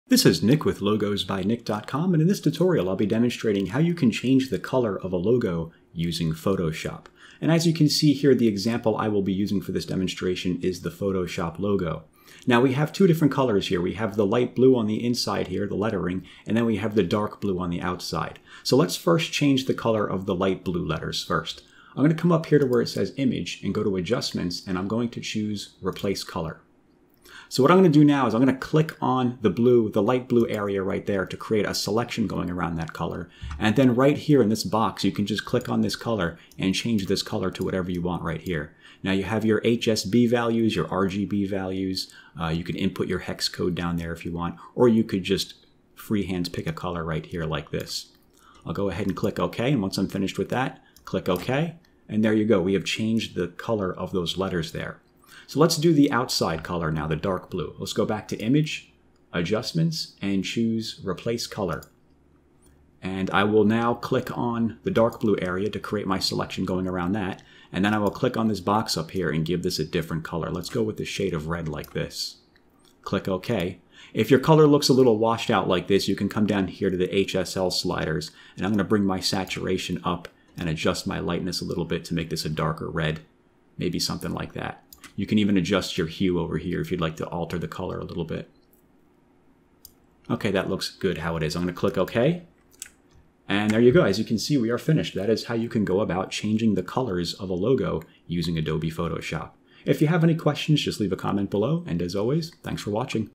How To Change A Logo Color In Photoshop CC 2022
If you only have flattened, rasterized copies of your logo (such as PNG and JPG) then you may find it difficult to make alterations such as color. In this tutorial we'll be using a workaround that will allow you to change a logo color in Photoshop without needing vector copies and without needing to use Illustrator.